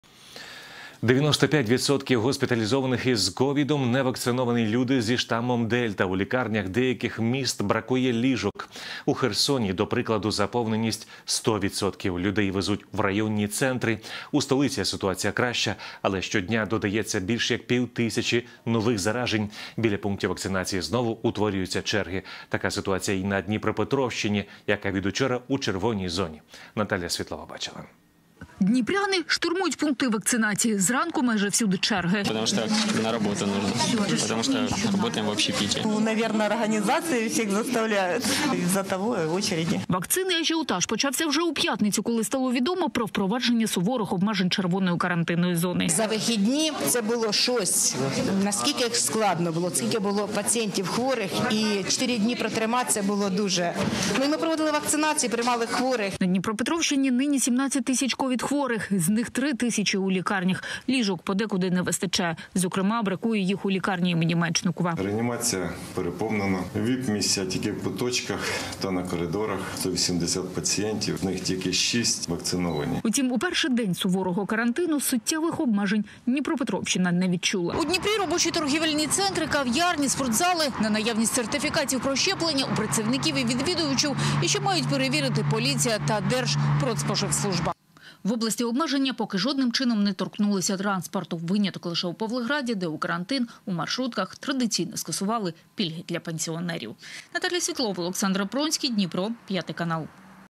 Дніпряни штурмують пункти вакцинації: чому виник ажіотаж та яка ситуація із захворюваністю
95% госпіталізованих із КОВІД- невакциновані люди зі штамом Дельта. У лікарнях деяких міст бракує лІжок. У Херсоні, до прикладу, заповненість 100%. Людей везуть у районні центри. У столиці ситуація краща, але щодня додається більш як пів тисячі нових заражень. Біля пунктів вакцинації знову утворюються черги. Така ситуація і на Дніпропетровщині, яка від учора у червоній зоні.